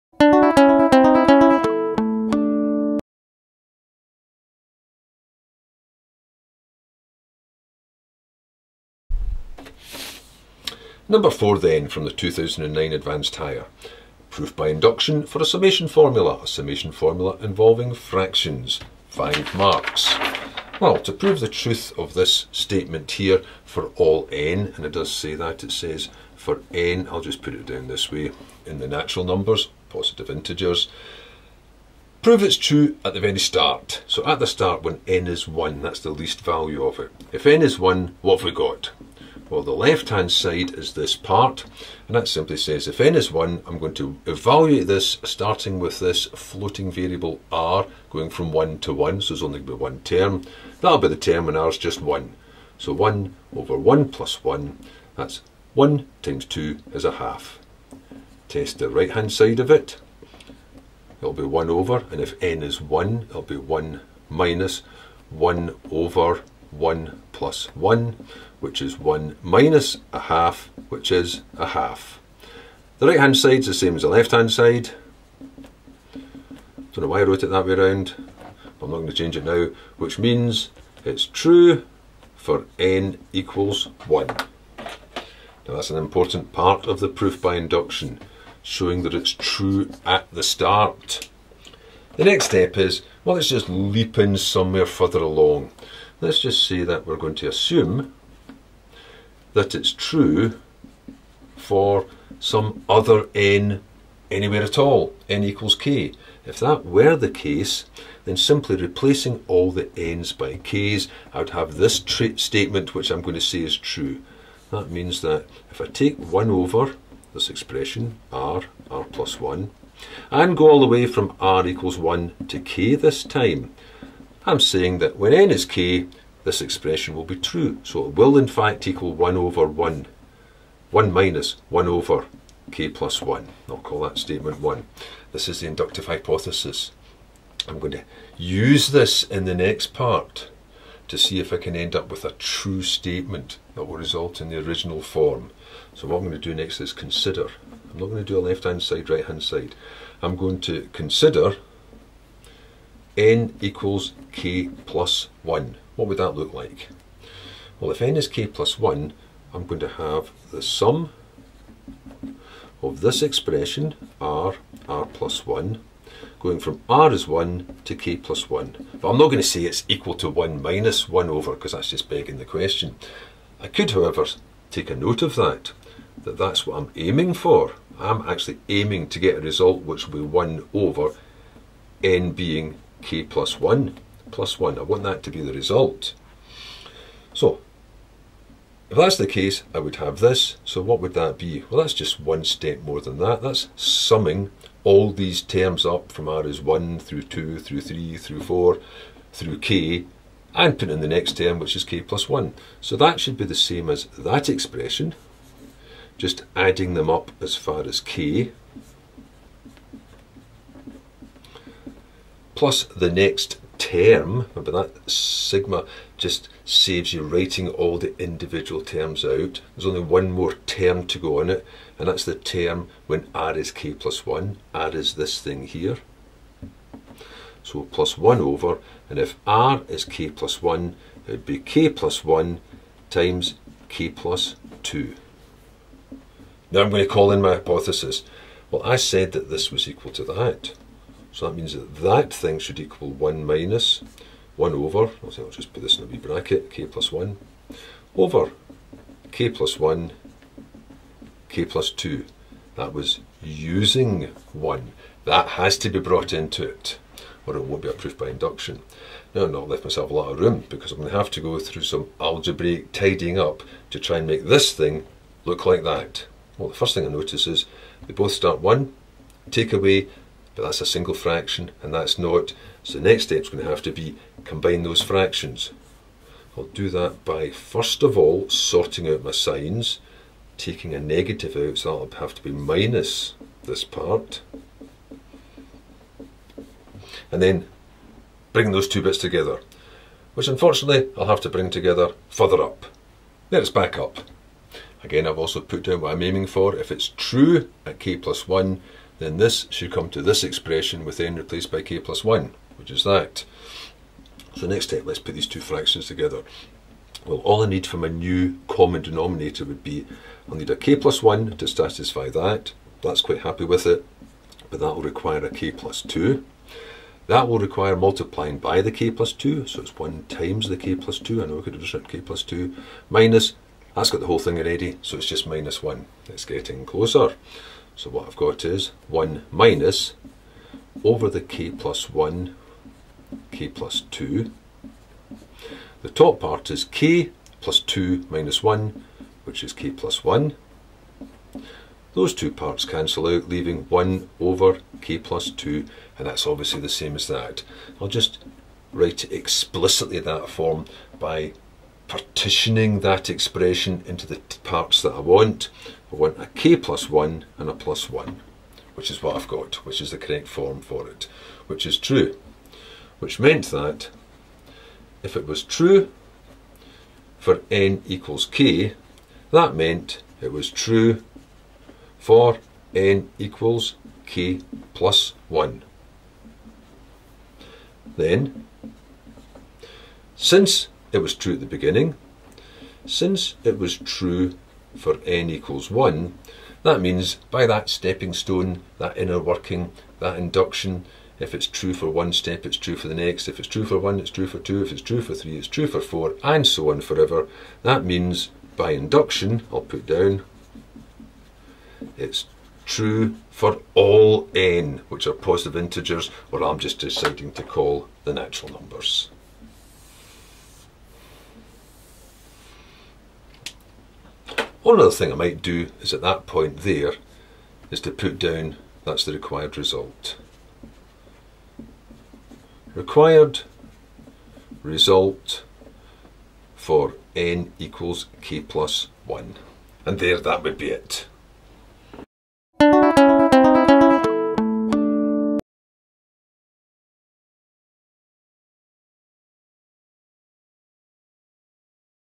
2009 SQA Advanced Higher Maths no.4 Proof by induction
Instructional exercise consisting of question 4 from
the 2009 SQA Advanced Higher Mathematics exam.

Proof by induction.
Summation formula involving fractional terms.